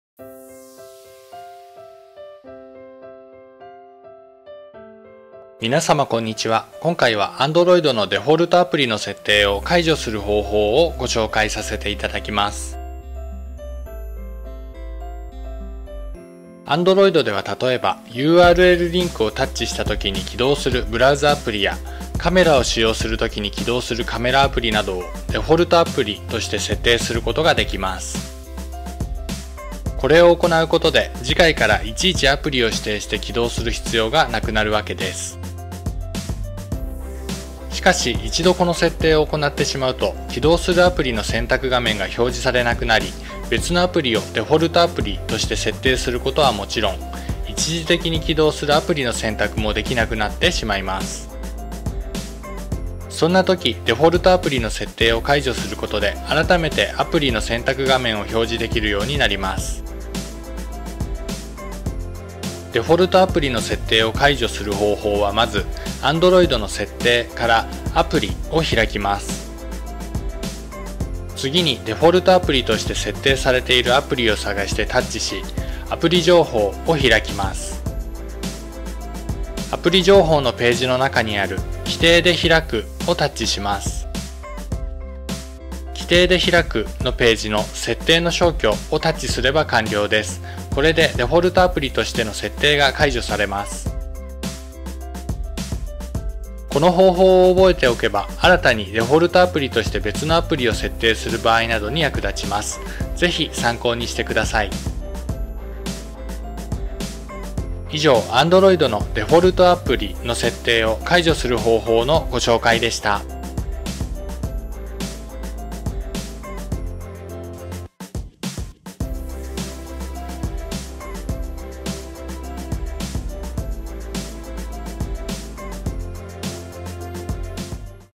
Android の “デフォルトアプリ” の設定を解除する方法
【今回の内容】
今回は、Android の “デフォルトアプリ” の設定を解除する方法をご紹介しています。

【海外ガジェットとアクセサリーのセレクトショップ・Jetstream SHOP】